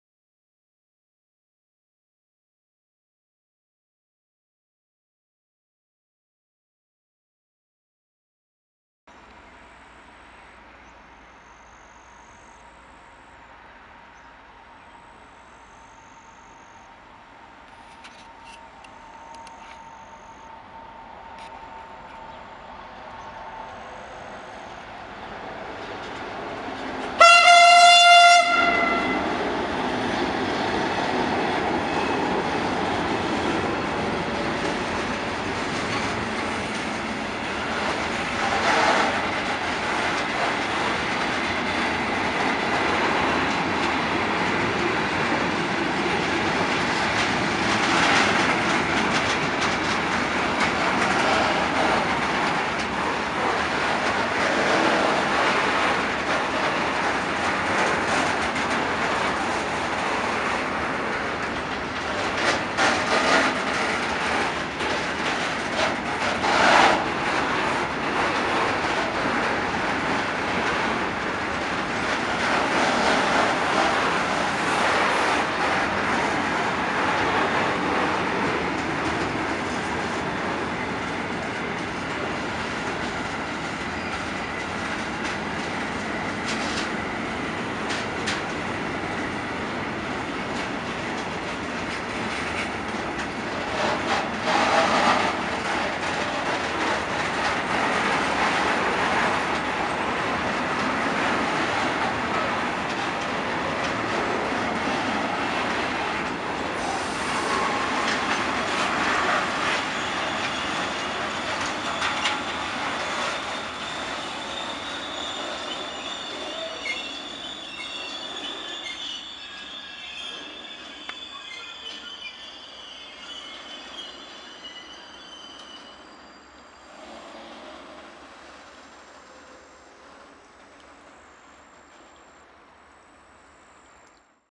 GRANDIOSO TEC DI SHUTTLEWISE CON G2000 02 SERFER...
...appena arrivato da Rotterdam/Venlo, con la E186 907 di Crossrail che vedete nelle inquadrature a inizio video, in trasferimento al Terminal Intermodale di Mortara.
Un saluto ai simpatici macchinisti che ormai sono miei fans :-) Ciao ragazzi!!!
Mortara, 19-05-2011 ore 17,45 circa